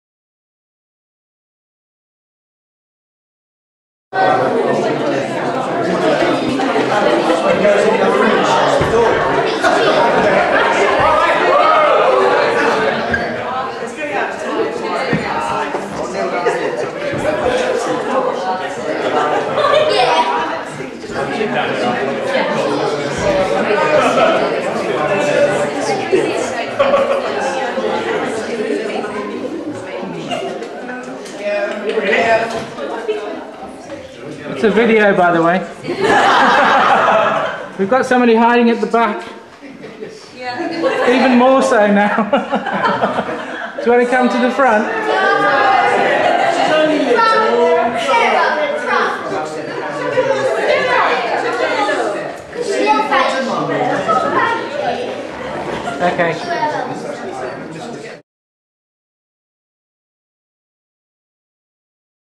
Trent April 08 Group Photo Setup
Setting up the entire group photo from the Trent park family meeting Saturday 19th April 2008